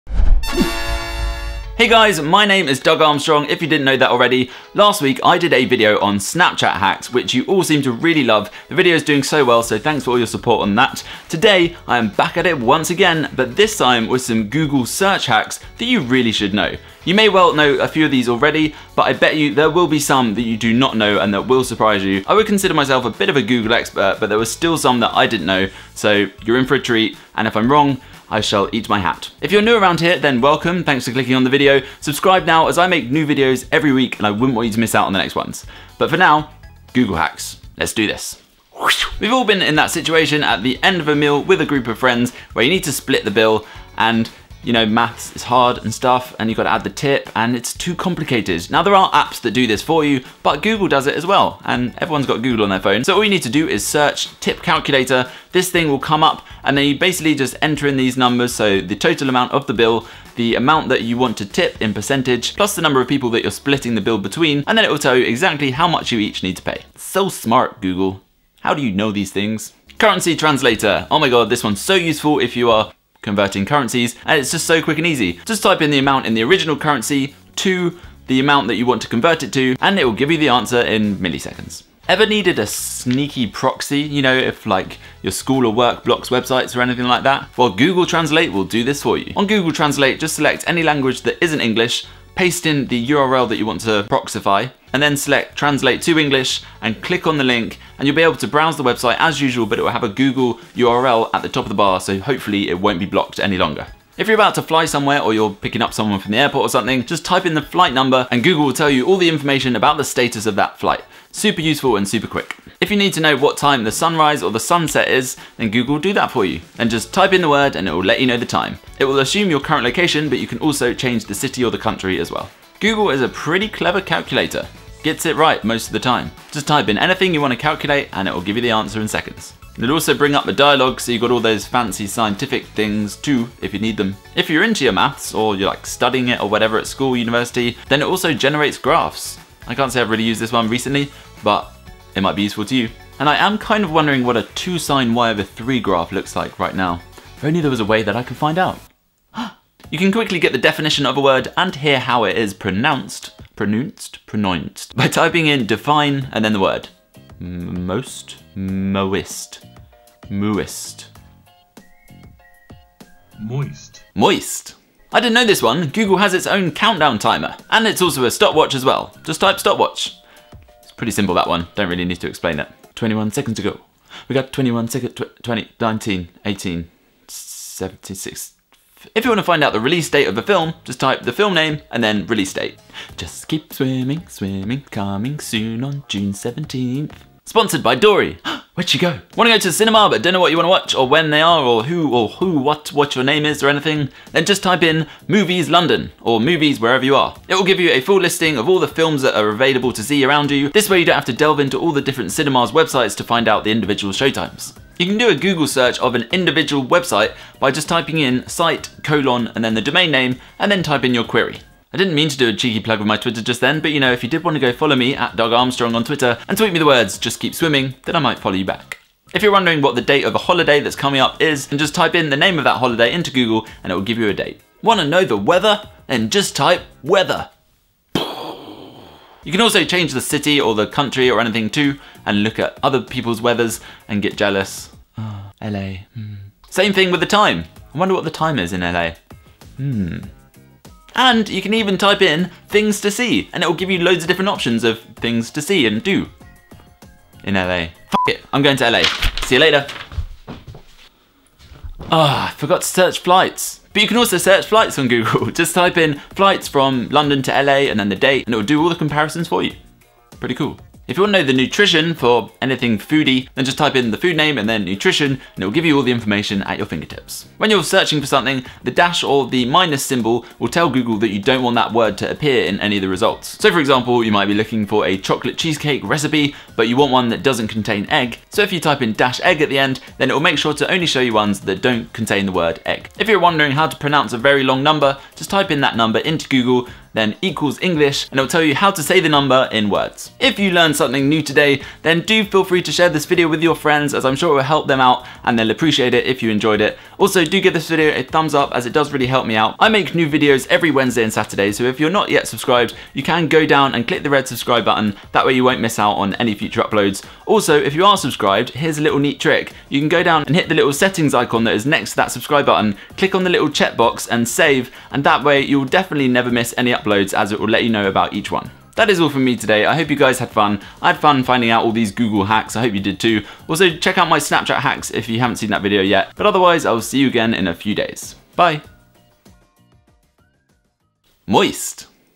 20 Google Hacks That You Really Should Know!
Hacks, tips & tricks for using Google Search, that you definitely need in your life! ➨ Thumbs up? :)

secret comment if you read this far: Moist 💦